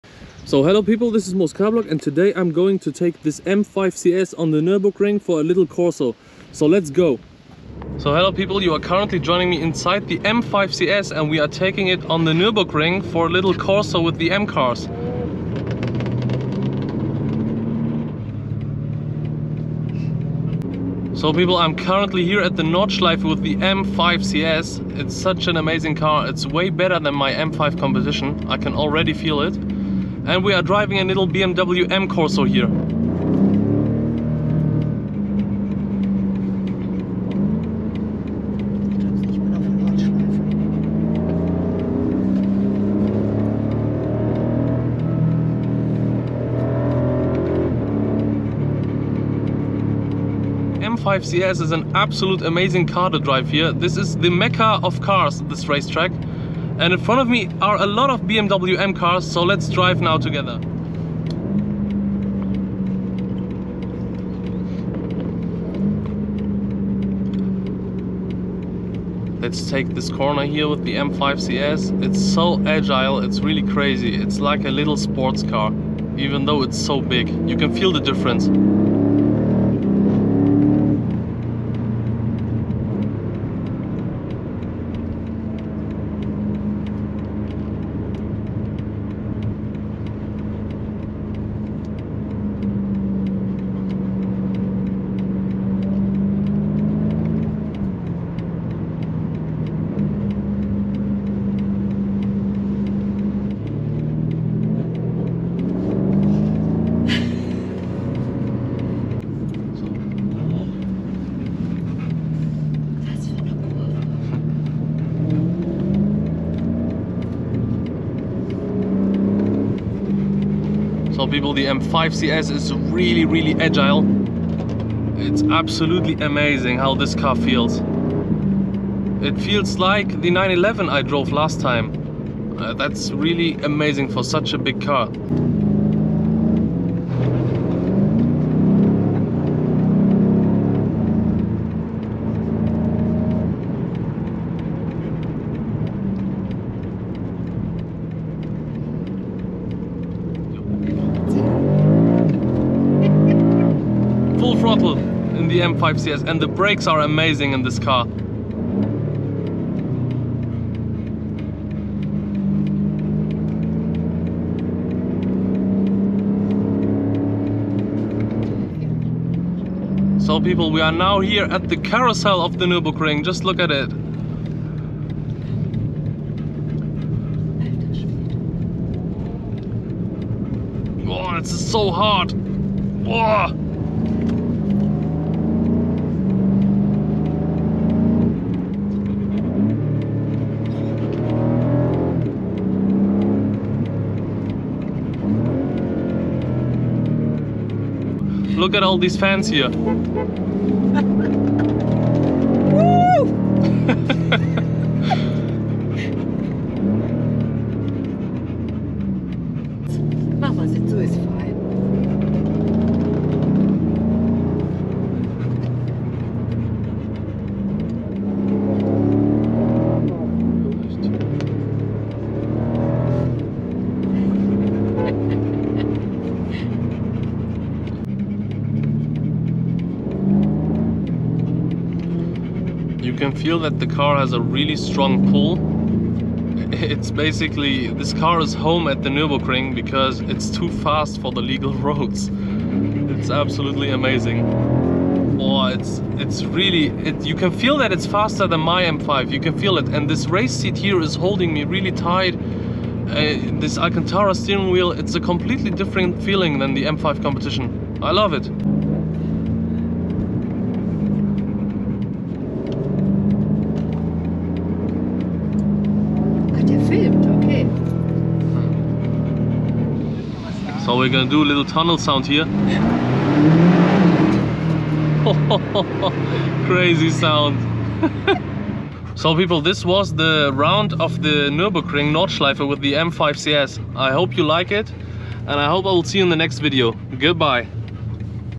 BMW M5CS Nürburgring POV!
Hello people I am Moscarblog and I drive and review the coolest and newest cars for you!

In this Video I am taking on the amazing 2022 Bmw M5CS and doing a little Corso on the Nürburgring

Don‘t forget to follow the Instagram to always be up to date!